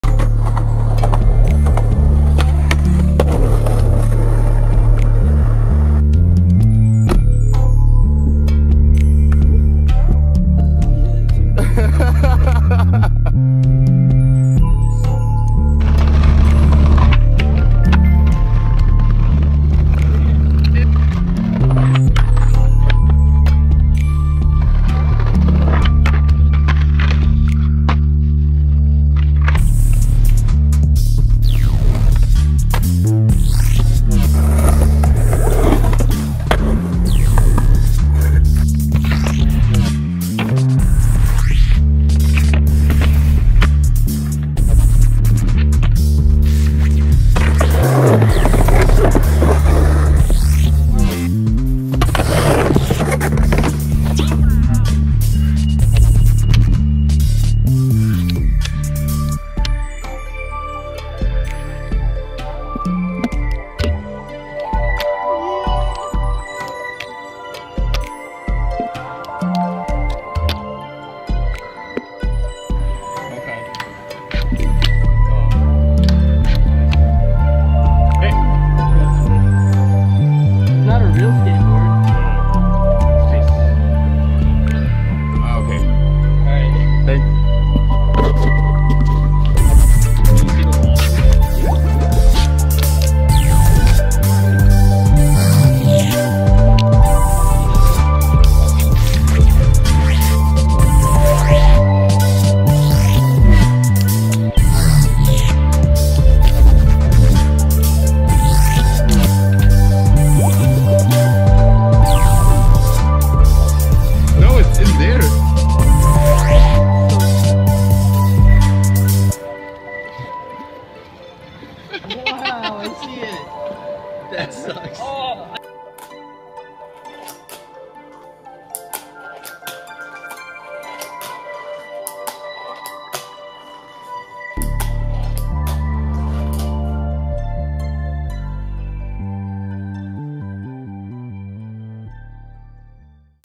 Spring Adventures
Fun with Harry, Drake, Shayne, and myself. We even got kicked out for bing at the mall! And some fun skating.